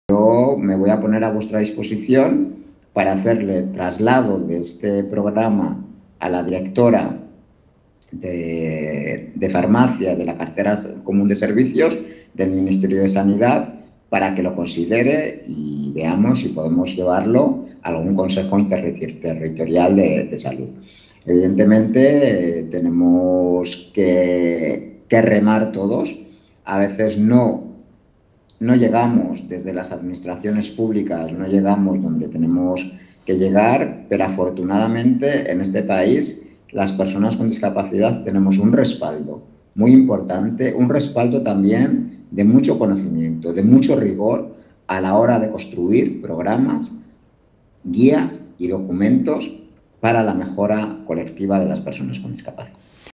Jesús Martín Blanco se compromete a llevar el programa de salud de Down al Ministerio de Sanidad
Jesús Martín Blanco, en nombre del Ministerio de Derechos Sociales y Agenda 2030, se comprometió a hacer llegar el programa de salud de Down España a la directora general de Farmacia del ministerio de Sanidad "para ver si podemos llevarlo al Consejo Interterritorial de Salud” y sacarlo adelante junto a las comunidades autónomas.